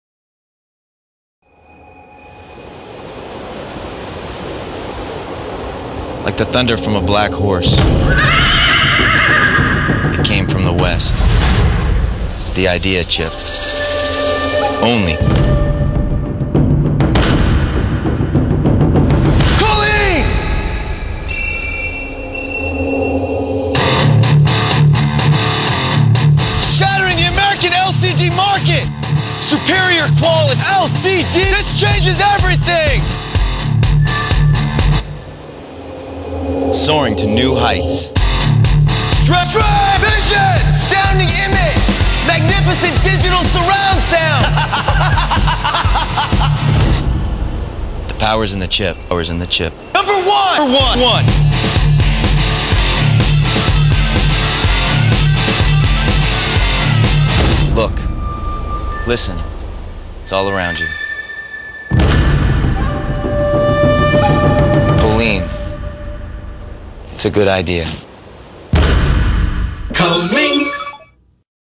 KOLIN COMMERCIAL
2005 Kolin Commercial starring Travis Aaron Wade shot in The Grand Canyon, AZ